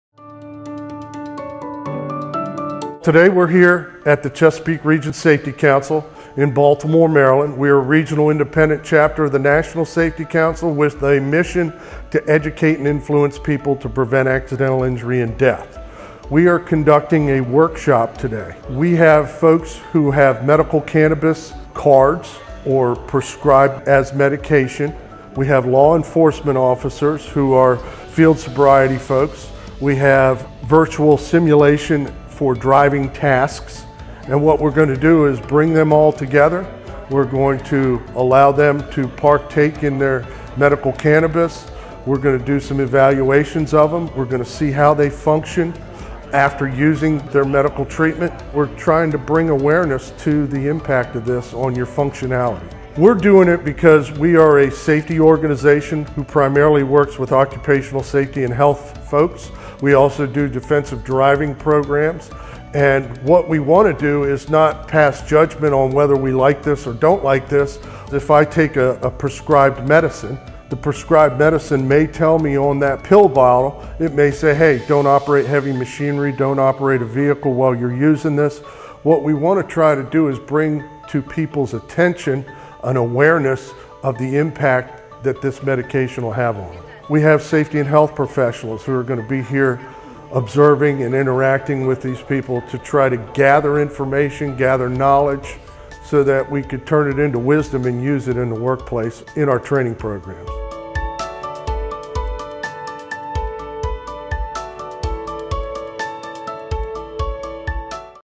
CRSC Cannabis Workshop David Interview FINAL 1
Thank you to the cannabis patients and our training partners - Maryland's Law Enforcement Drug Recognition Experts, Draeger Inc., Bloom Medicinals and Diagnostic Driving, Inc!  Chesapeake Region Safety Council recently conducted a cannabis use workshop at our training center as we prepare to conduct Reasonable Suspicion and Risk Management classes for employers.